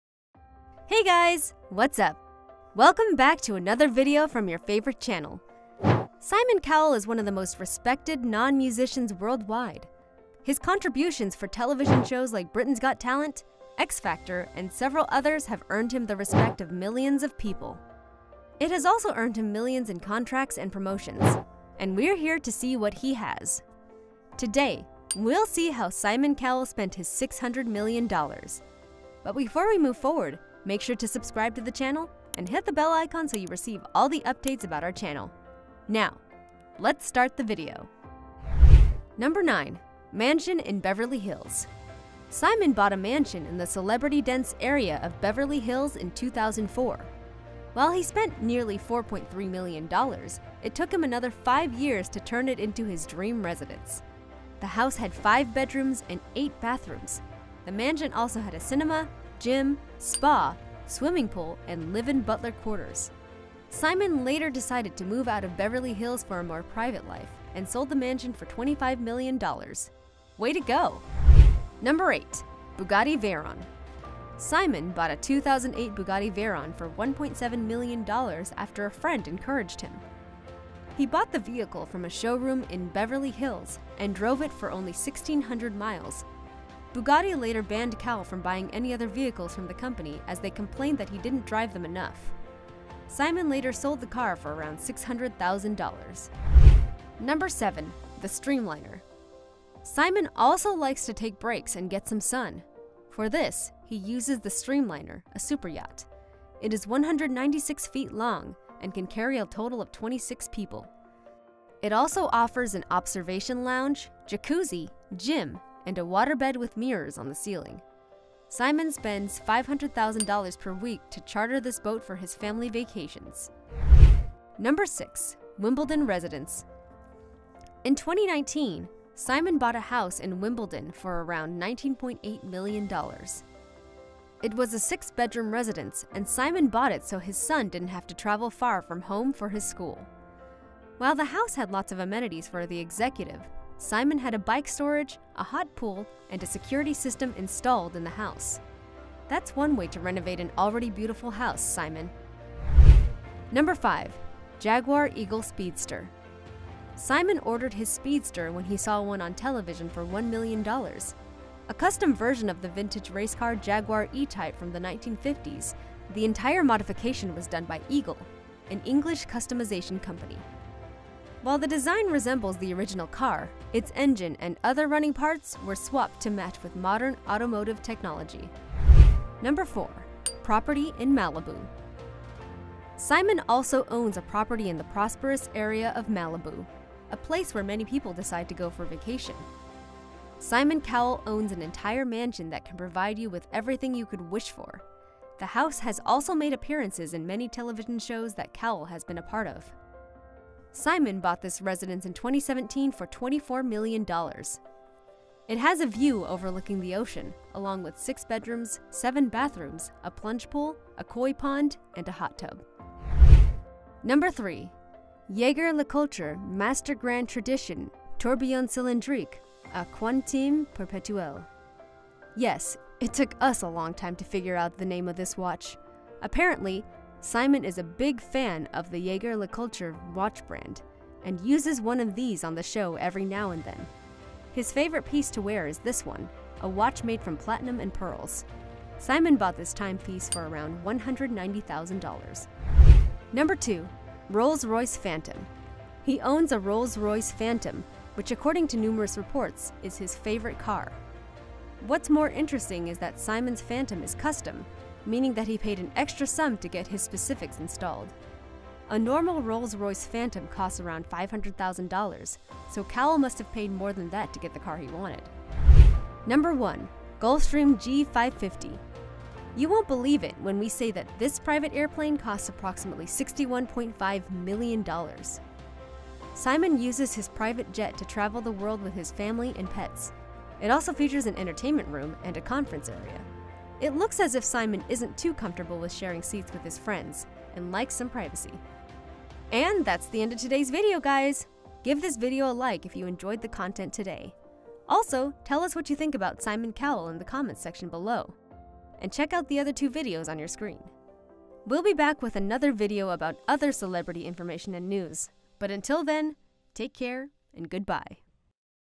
How Simon Cowell Spends $600 Million!
Hello there, and welcome back to Luxistic! Simon Cowell has made a name for himself in the music and entertainment industry. While he's not being a judge on the X Factor or managing artists, he's surely spending time with his family. And with a net worth of $600 million, he can definitely afford the finer things in life. With that being said, in today's video, we will be taking a closer look at how he spends his millions!